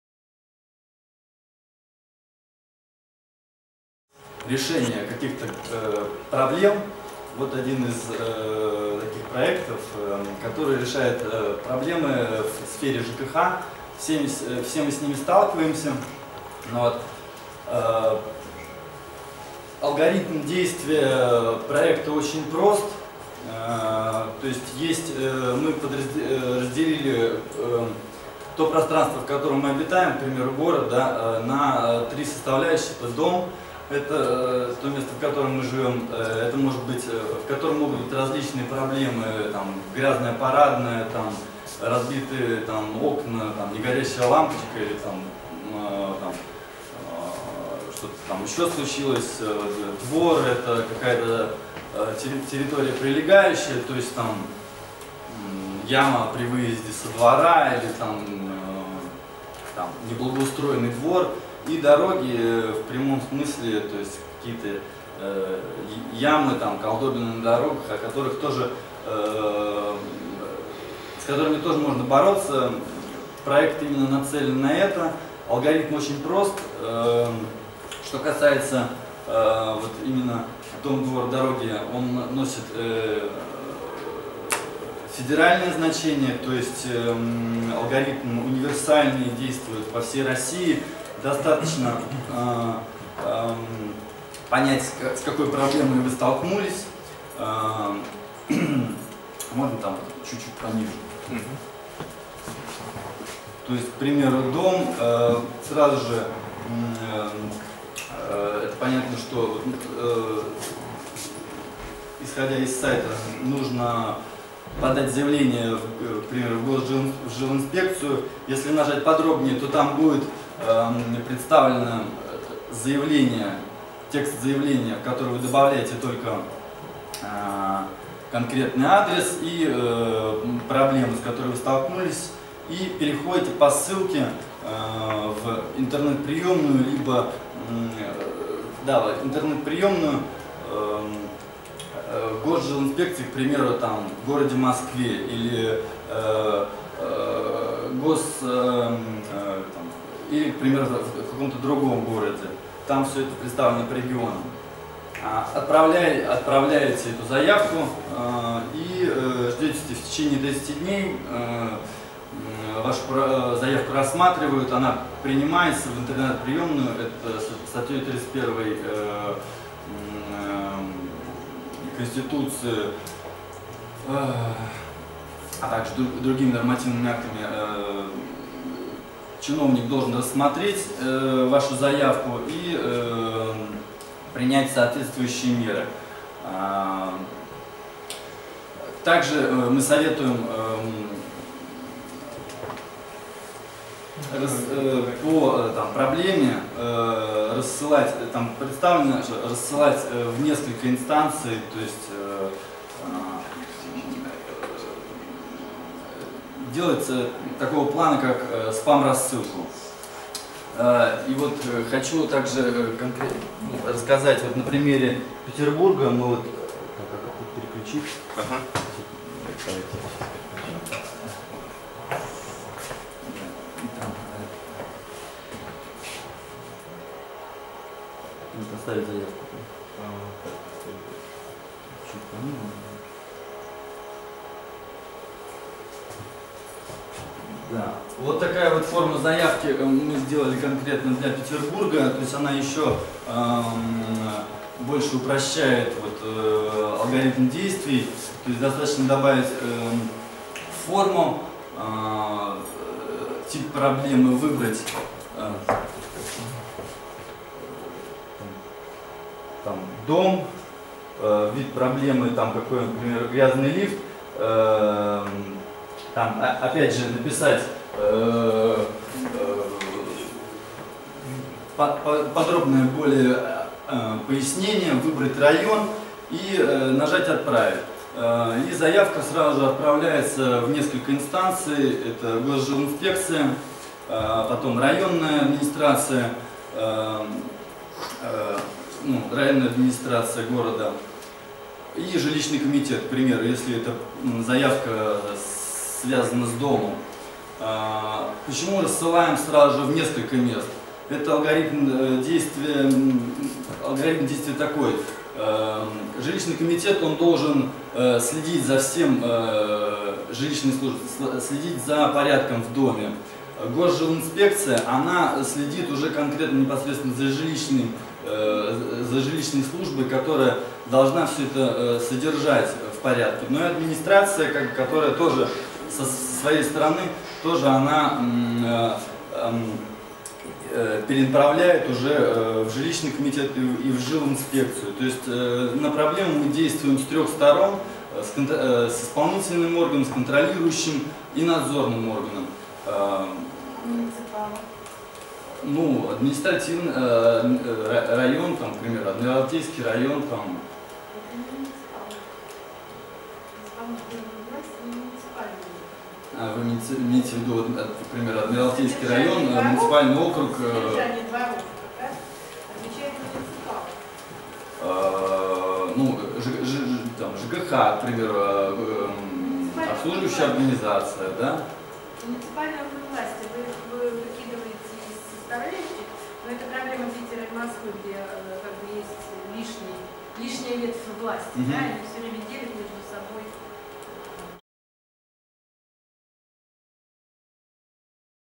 Владимир Бондаренко о проекте Дом.Двор.Дороги [Идея]
Владимир Бондаренко представляет идею проекта "Мобильная версия Дом.Двор.Дороги" на ТеСТ-кэмп Петербург